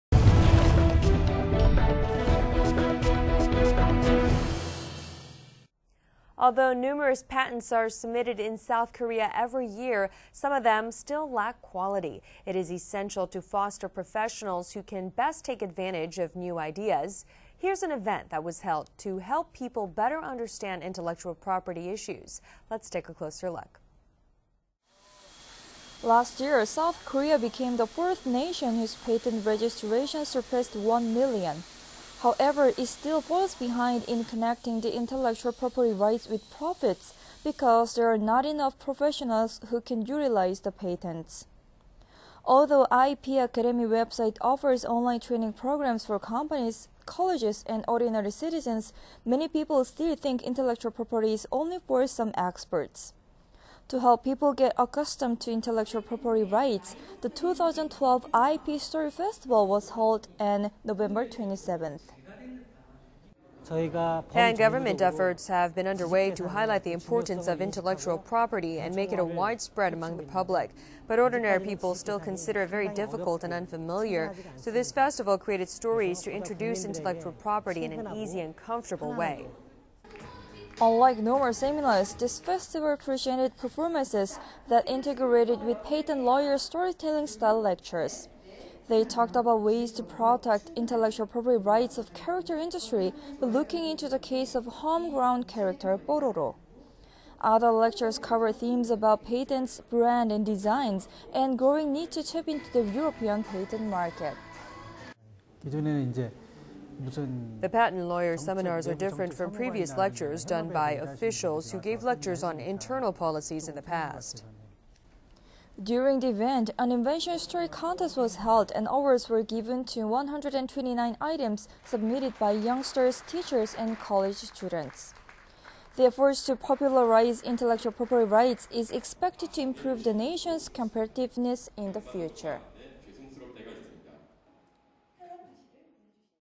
Intellectual Property Story Festival held
Anchor:
Although numerous patents are submitted in South Korea every year, some of them still lack quality. It is essential to foster professionals who can best take advantage of new ideas. Here's an event that was held to help people better understand intellectual property issues. Let's take a look.

Reporter:
Last year, South Korea became the fourth nation whose patent registrations surpassed the one million mark. However, it still falls behind in connecting intellectual property rights with profits because there are not enough professionals who can utilize these patents. Although the IP academy website offers online training programs for companies, colleges and ordinary citizens, many people still think intellectual property is strictly for experts. To help people get accustomed to intellectual property rights, the '2012 IP Story Festival' was held on November 27.

Interview- Song Sang-yeop, official at Korea Invention Promotion Center
Pan-government efforts have been underway to highlight the importance of intellectual property and make it widespread among the public, but (ordinary people) still consider it very difficult and unfamiliar. So (this festival) created (stories) to introduce (intellectual property) in an easy and comfortable way.

Unlike normal seminars, this festival presented performances that integrated patent lawyers' story-telling style lectures. They talked about ways to protect the intellectual property rights of the character industry by looking into the case of the home-grown character "Ppororo." Other lectures covered themes of patents, branding and design and the growing need to tap into the European patent market.

Interview - Kim Jong-hyun, senior executive at engineering firm
The patent lawyers' seminars are different from previous lectures done by officials, who gave lectures on internal policies in the past.

During the event, an invention story contest was held and awards were given to 129 items submitted by youngsters, teachers and college students. The effort to popularize intellectual property rights is expected to improve the nation's competitiveness in the future.

스토리가 있는 지식재산 축제 개최

[앵커멘트]
우리나라 지식재산 창출은 양적인 측면에서는 우수한 편이지만, 질적인 측면에서는 많이 부족합니다. 지식재산을 잘 활용할 수 있는 인재를 육성하는 것이 시급한데요. 이에 지식재산을 더욱 쉽고 친근하게 접할 수 있는 행사가 열렸습니다.

[리포팅]
우리나라는 지난해 세계에서 4번째로 특허 등록 100만 건을 돌파했을 정도로 지식재산 창출에 적극적입니다.

하지만 이를 수익으로 연결하는데는 실적이 미미합니다.

지식재산을 잘 활용할 수 있는 인재가 부족하기 때문입니다.

국가지식재산교육 사이트에서 기업과 대학, 일반인들을 위한 맞춤형 온라인 교육 프로그램을 제공하고 있지만, 여전히 지식재산을 일부 전문가들의 분야로만 생각하는 사람들이 많습니다.

이에 지식재산을 더욱 쉽고 가까운 것으로 느낄 수 있도록 돕는 '2012 지식재산 스토리 페스티벌'이 지난 27일 엘타워에서 개최됐습니다.

[인터뷰 - 송상엽 교육기획팀장 / 한국발명진흥회]
~ 저희가 범정부적으로 지식재산의 중요성을 인식하고 대중화에 힘쓰고 있는데 아직까지는 (대중들에게) 지식재산이 상당히 어렵고 친근하지가 않습니다. 그래서 (스토리를 엮어) 국민들에게 친근하고 편안하게.

기존 세미나와는 달리 지식재산을 소재로 한 공연과 함께 스토리를 접목시킨 변리사들의 강연이 펼쳐졌습니다.

국산 캐릭터 뽀로로의 사례를 통한 캐릭터 산업의 지식재산권 보호 방법과 국내 기업들의 유럽 특허 출원 증대의 필요성 등  특허와 상표, 디자인과 관련된 6개의 주제로 진행됐습니다.

[인터뷰 - 김종현 이사 / 엔지니어링 업체]
~ 기존에는 내부 정책을 다루는 사무관들이 (강연을) 많이 했는데 현업에서 일하시는 분들(변리사)이 와서 세미나를 해주니까 조금 다른 것 같아요.

또한 이 날, 우수 발명 스토리를 뽑는 '2012 대한민국 발명스토리 콘텐츠 공모전 시상식'도 개최돼 청소년, 교원, 대학생 부문에서 총 129점이 수상했습니다.

쉽고, 재미있고, 친숙하게 지식재산권을 접하기 위한 노력이 국가 경쟁력 향상에도 도움이 될 것으로 기대됩니다.